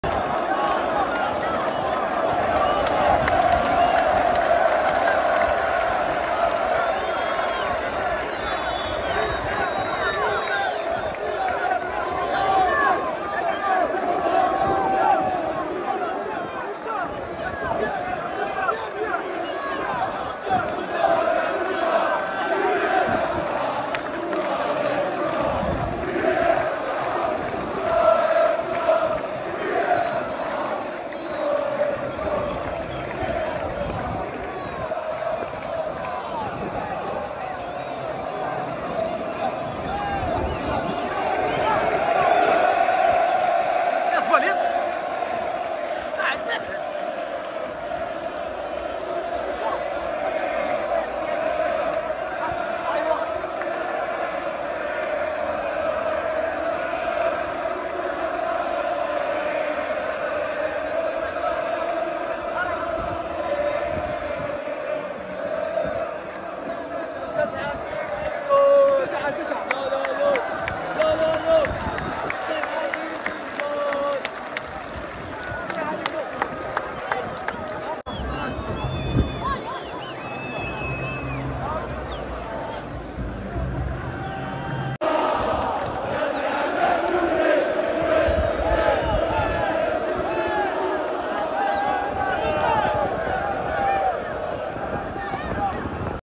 شاهد إحتفال سفيان بوشار بالكأس مع جمهور بلوزداد في مباراة الوداع قبل عودته لفريقه العربي الكويتي 🇰🇼
شاهد كيف إحتفل القائد سفيان بوشار بالكأس مع جمهور بلوزداد في مباراة الوداع قبل عودته لفريقه العربي الكويتي 🇰🇼